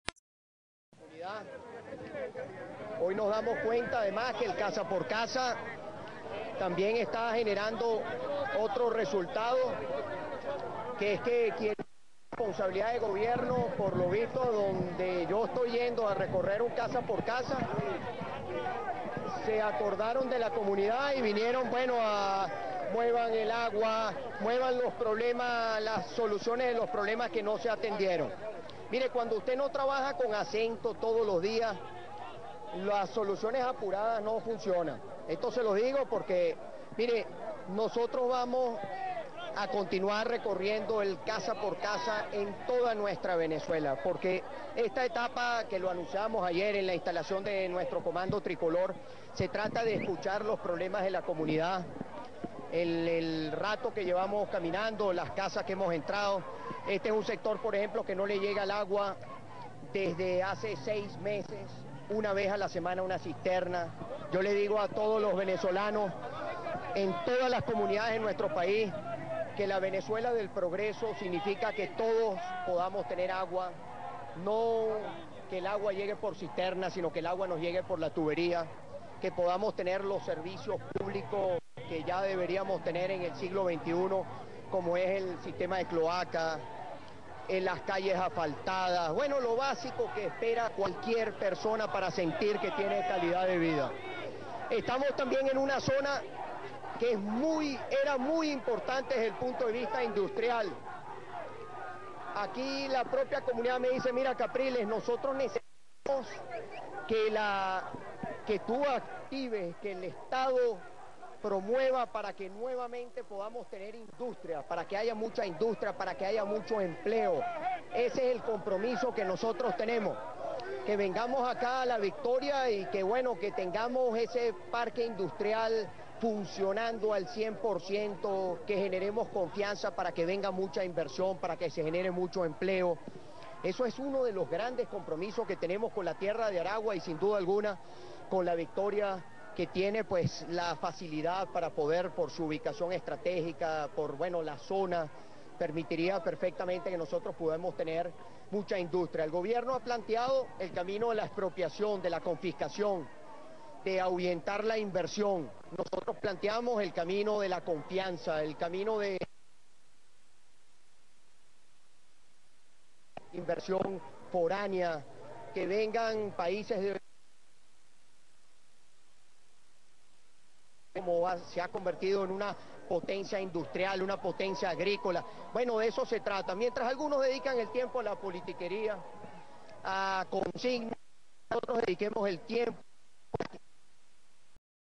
Capriles R. en La Victoria
Casa por Casa en La Cictoria el candidato presidencial Henriqque Capriles